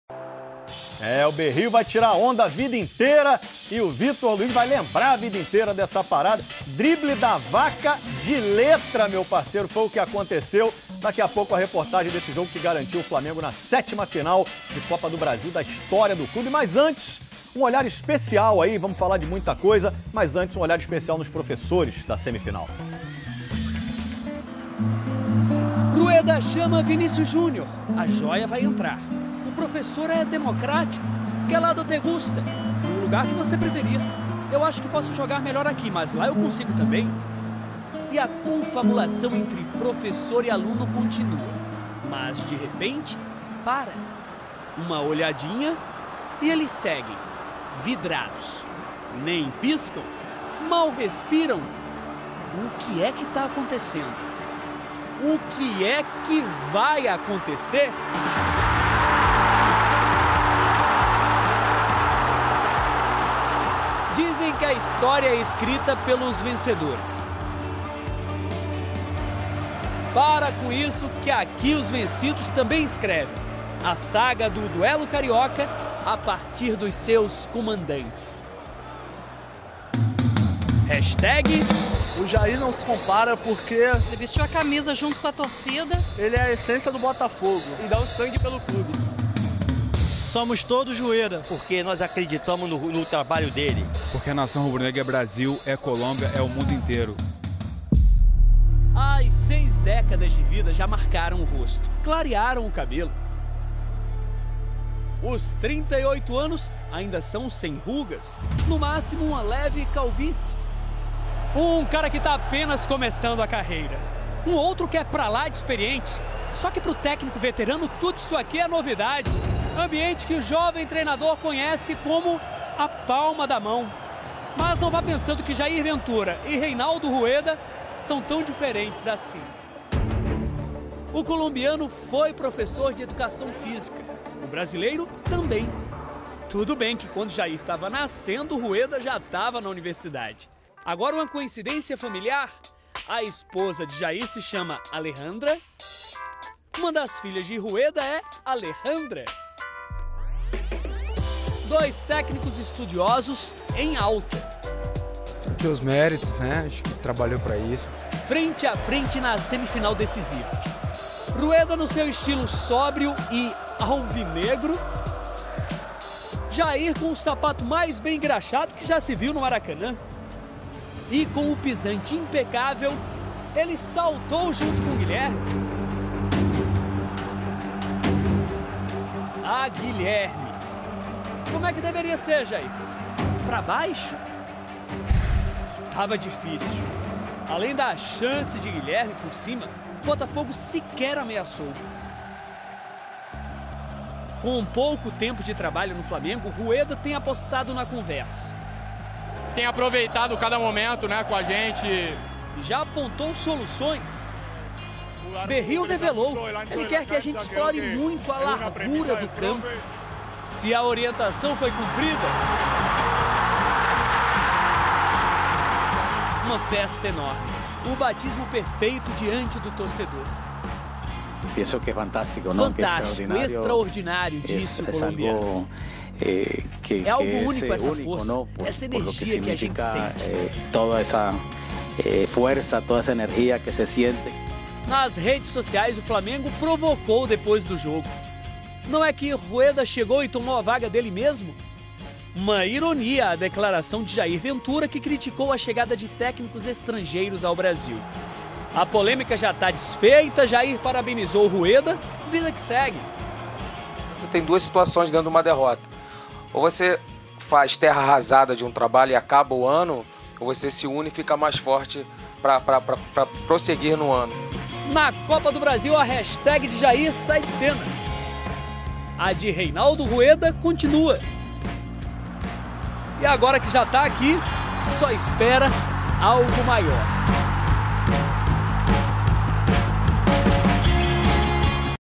Reportagem - Duelo entre Jair Ventura-Botafogo e Reinaldo Rueda-Flamengo [23-08-2017]-
História entre o duelo dos técnicos Jair Ventura e Reinado Rueda.